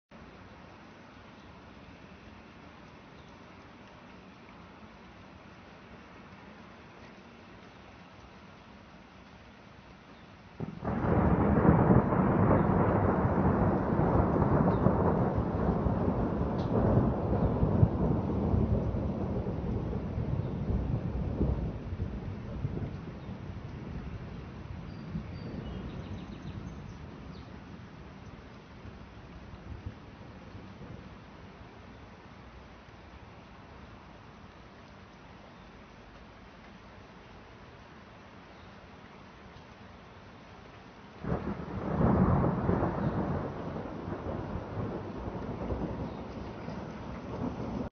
Portland Thunderstorm 5-28-2014 part 2
More the the thunderstorm that went through Portland, Oregon.